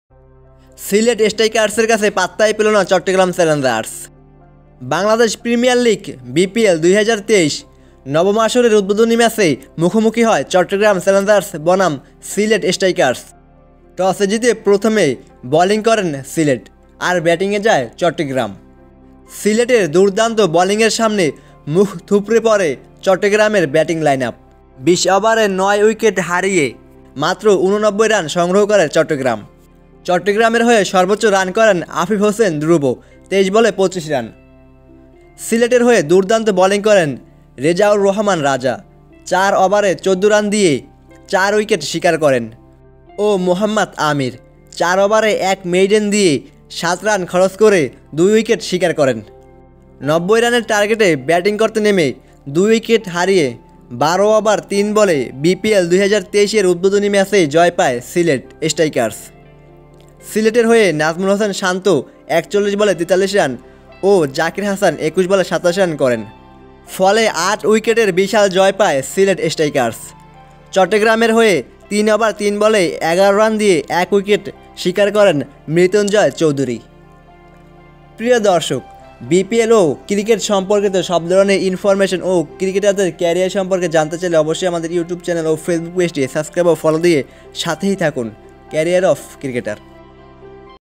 সিলেটের সাথে পাত্তাই পেলো না চট্টগ্রাম। বিপিএল ২০২৩ উদ্ভোদনী ম্যাচ। Sylhet vs Chattogram
ক্রিকেটারদের সব ফর্মেটের পারফম্যান্স সম্পর্কে জানানো হবে ইনশাআল্লাহ

সিলেটের সাথে পাত্তাই পেলো না চট্টগ্রাম।

বাংলাদেশ প্রিমিয়ার লিগ বিপিএল ২০২৩ এর নবম আসরে সিলেট স্ট্রাইকার্সের মুখোমুখি হয় চট্টগ্রাম চ্যালেঞ্জার্স।  টসে জিতে বোলিং করার সিদ্ধান্ত নেন সিলেট। আর ব্যাটিংয়ে যায় চট্টগ্রাম।
সিলেটের বোলিংয়ের সামনে মুখ থুপরে পরে চট্টগ্রামের ব্যাটিং লাইন আপ।  ২০ ওভারে ৮ উইকেট হারিয়ে মাত্র ৮৯ রান করে চট্টগ্রাম।  চট্টগ্রামের হয়ে সর্বোচ্চ রান করেছেন আফিফ হোসেন দ্রুব ২৩ বলে ২৫ রান।  সিলেটের হয়ে সর্বোচ্চ বোলিং করেছেন রেজাউর রহমান রাজা ৪ ওভারে ১৪ রান দিয়ে ৪ উইকেট শিকার করেন ও মোহাম্মদ আমির ৪ ওভারে ১ মেইডেন দিয়ে ২ উইকেট শিকার করেন ।৯০ রানের টার্গেটে ব্যাটিং করতে নেমে ২ উইকেট হারিয়ে ১২.৩ বলে বিপিএল ২০২৩ এর উদ্ভোদনী ম্যাচে জয় পায়।  সিলেটের হয়ে সর্বোচ্চ রান করেছেন নাজমুল হোসেন শান্ত ৪১ বলে ৪৩ রান করেছেন ও জাকির হাসান ২১ বলে ২৭ রান করেছেন।